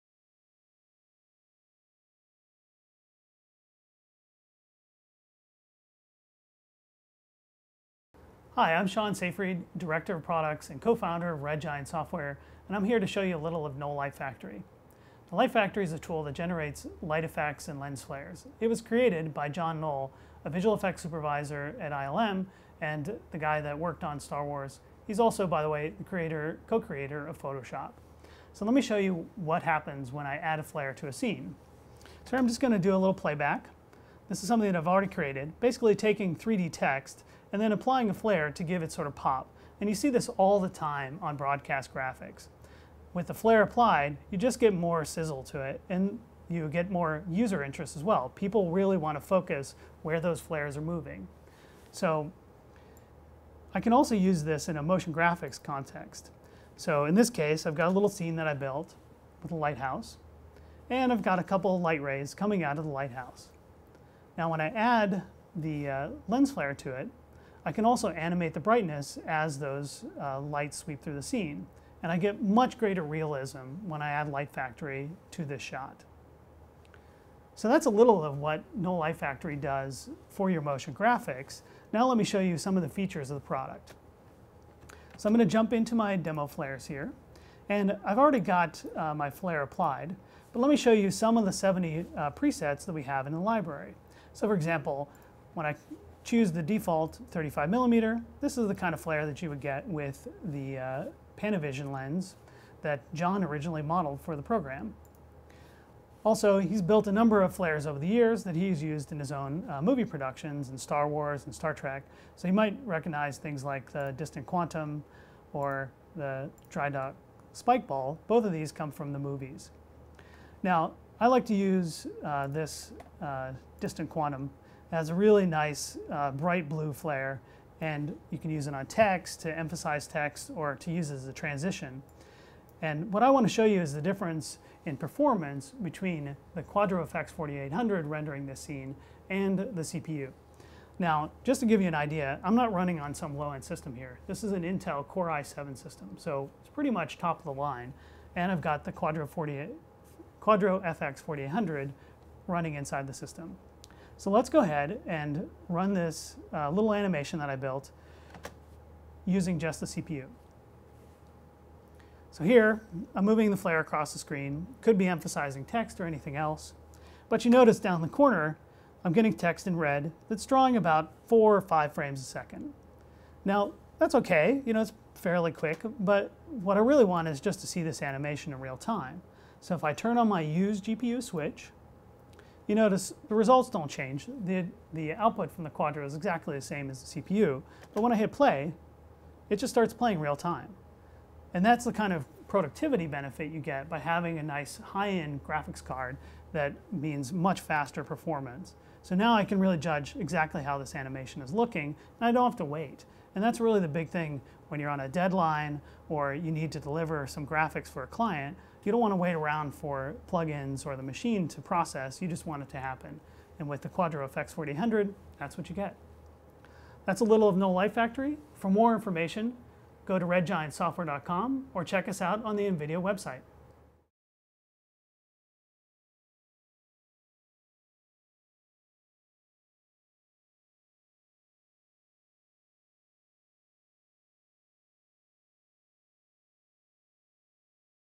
Knoll Light Factory Pro with NVIDIA Quadro GPUs
A brief overview of Knoll Light Factory Pro by Sean Safreed, Director of Products and Co-founder of Red Giant software.  He shows how you can easily add high-fidelity simulations of light effects called flares, which simulate reflections from a very bright light source as seen through a camera's lens.